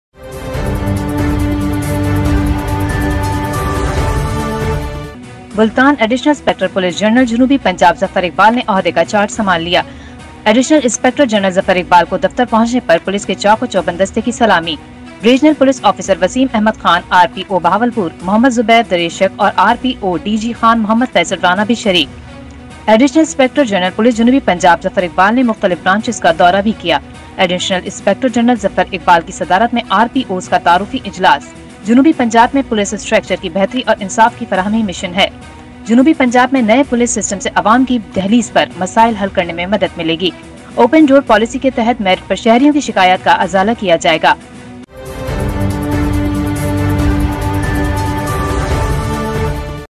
ملتان: ایڈیشنل انسپکٹر پولیس جنرل جنوبی پنجاب ظفر اقبال نے عہدے کا چارج سنبھال لیا ملتان: ایڈیشنل ا
Great news HD TV 30th September 2020 🇵🇰📺🇵🇰